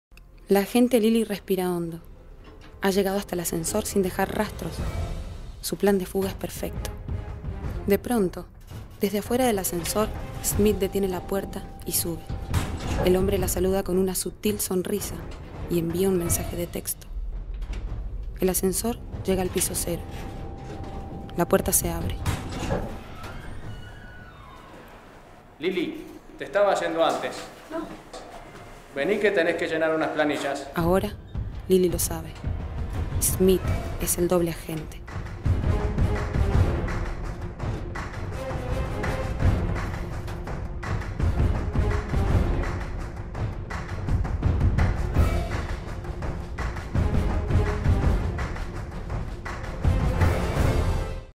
SPOT FERIA DEL LIBRO - Espía
Para la Feria del Libro 2013 realizamos una serie de spot, que muestran breves situaciones de gente común que ama la lectura y la ha incorpora a cada momento de su vida.